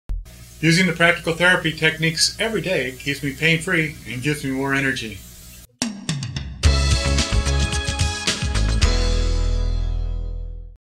Neck and Shoulder Pain as a Photographer
As a photgrapher and videographer John suffered from neck and shoulder pain from hours behind the camera and editing at his computer. With techniques from PracticalTherapy4U he eliminated his pain and keeps it away. See our "2Min2 a Better Neck" video to avoid pain in your neck and shoulders.